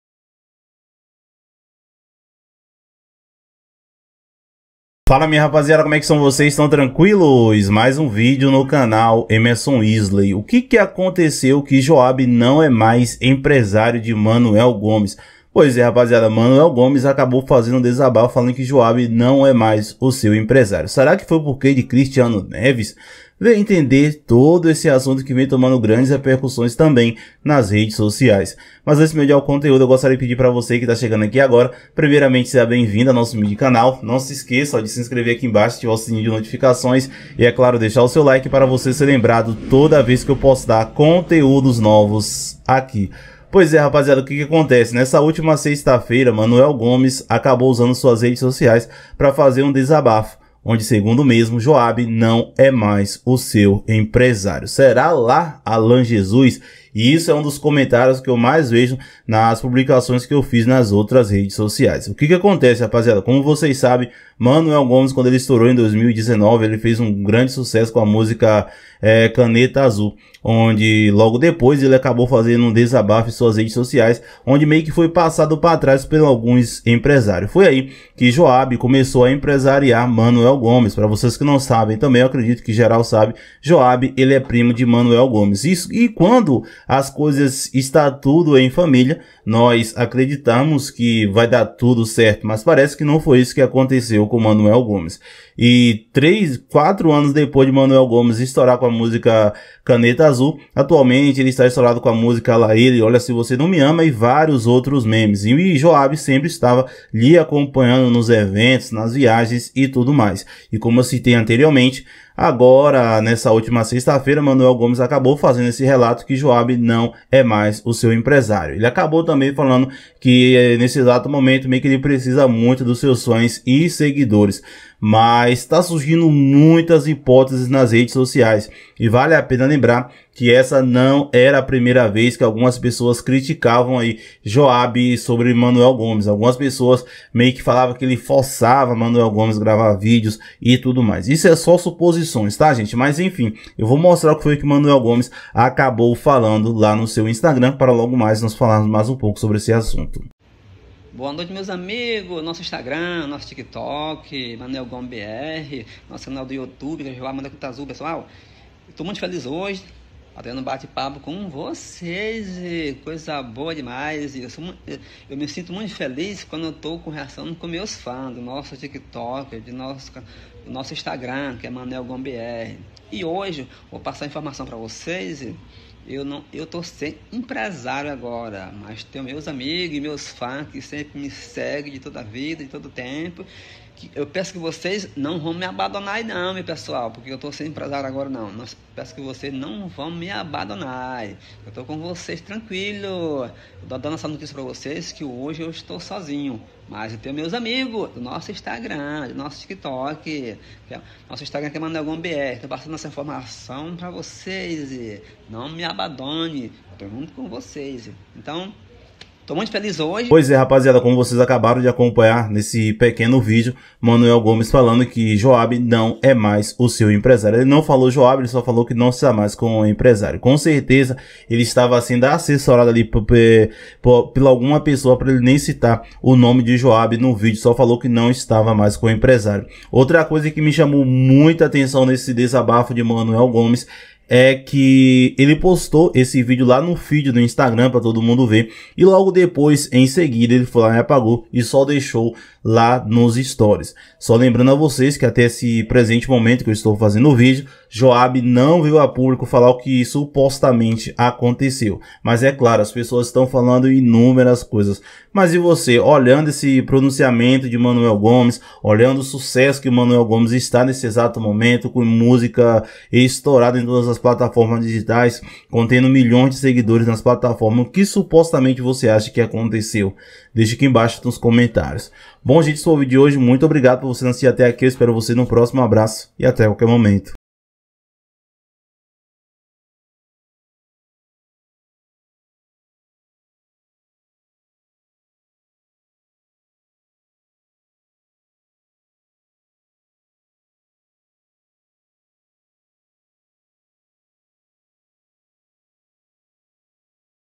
OQUE ESTÁ ACONTECENDO? MANOEL GOMES TOMA ATITUDE E ROMPE COM EMPRESARIO JOAB ! SAIBA OQUE ACONTECEU
MANOEL GOMES DESABAFA E DIZ QUE JOAB NÃO É MAIS SEU EMPRESÁRIO